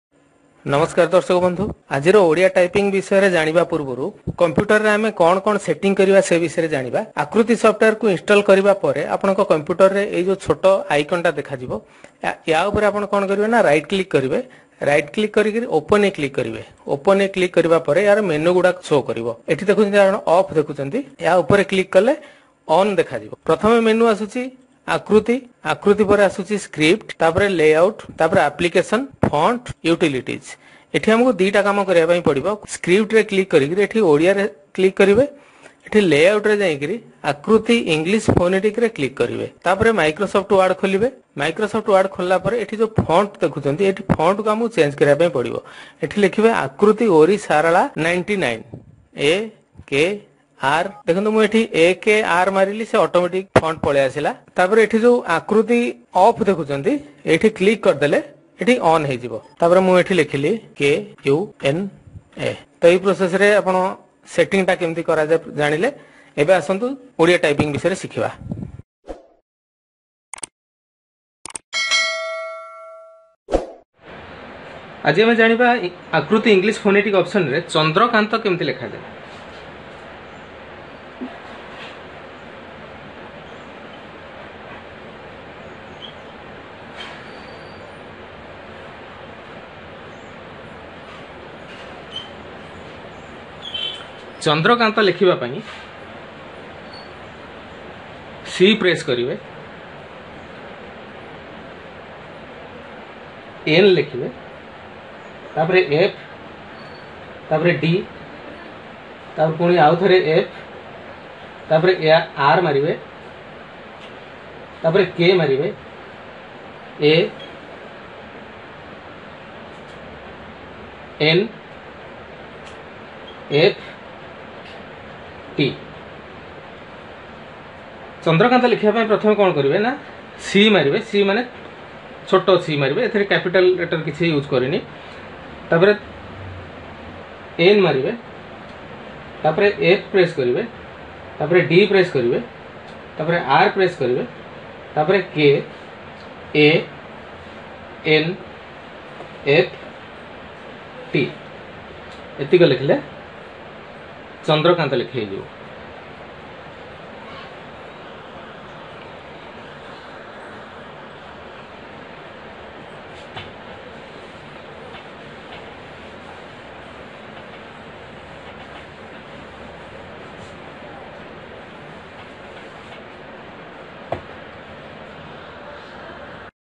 ଚନ୍ଦ୍ରକାନ୍ତ କେମିତି ଲେଖାଯାଏ ? - How to write CHANDRAKANT in Odia (Akruti English Phonetic)
ଓଡ଼ିଆ ଟାଇପିଙ୍ଗ PDF Download କରିବା ପାଇଁ ତଳ ଲିଙ୍କରେ କ୍ଲିକ କରନ୍ତୁ (Rs 65/-)